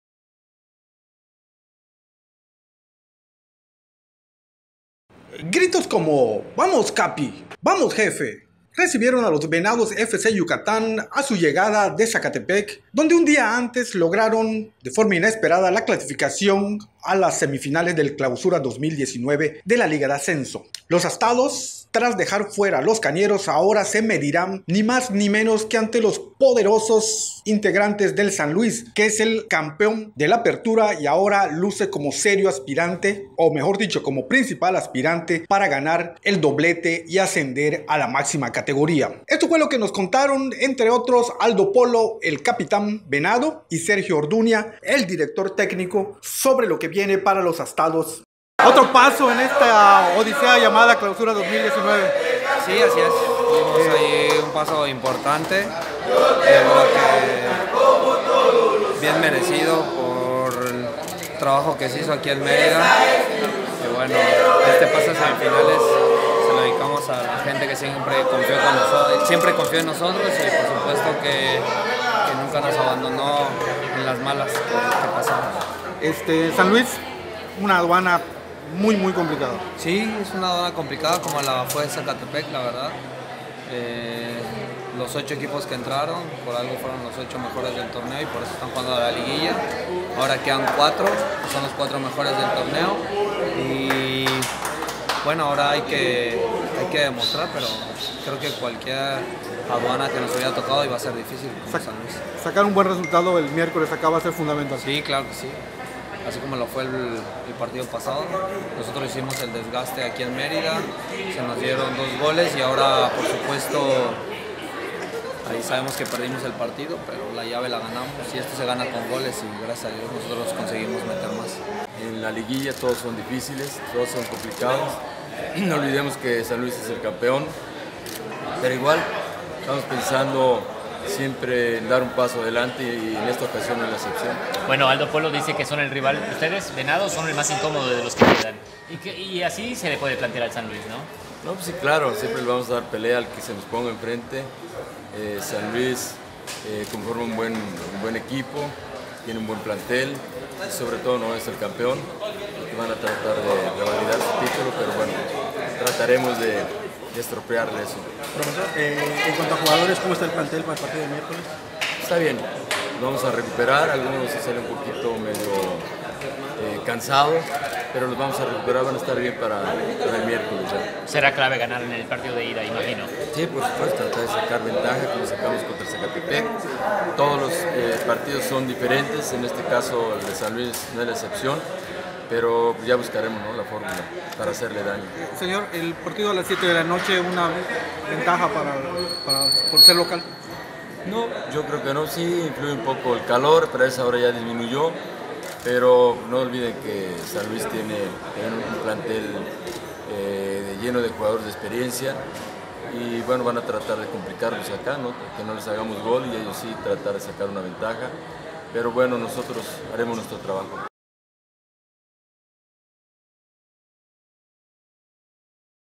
Los Venados, a darlo todo en otra eliminatoria de gente grande
Los Venados FC Yucatán llegaron a Mérida tras eliminar a Zacatepec en los cuartos de final del Clausura 2019. Ahora, a preparar la que sigue: enfrentar en semifinales al superlíder San Luis, abriendo la serie en casa el miércoles.

Aldo Polo, el capitán, y Sergio Orduña, el director técnico, hablaron con el Diario en el aeropuerto